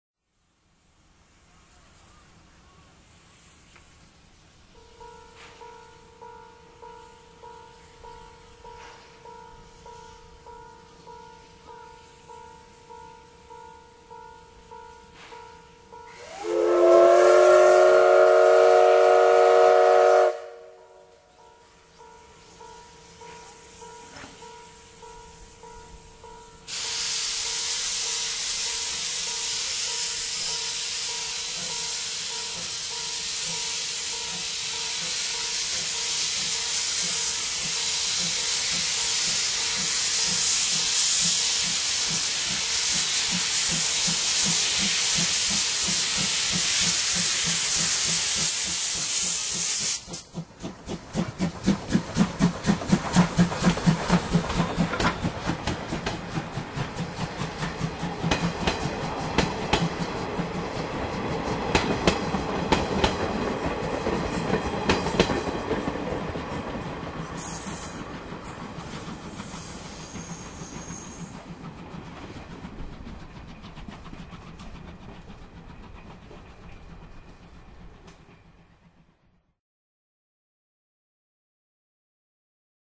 【音鉄】真岡鐵道 SLもおか号 上り 久下田駅発車の音風景 １１月４日
2013年11月4日収録　真岡鉄道久下田駅にて撮影
蒸気機関車の発車です！

新規導入したマイクの使い方にも段々習熟してきました！

ZOOM H2n にて収録後、SoundEngine Freeにてノーマライズ処理を行っています。